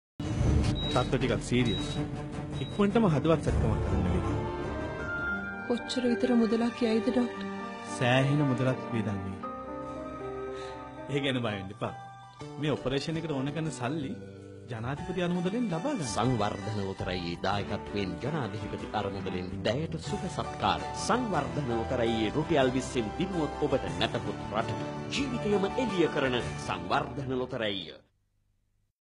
President's Fund
DLB has made tremendous contribution to the Sri Lankan nation through millions of Rupees as prizes and millions as profit to the President’s Fund.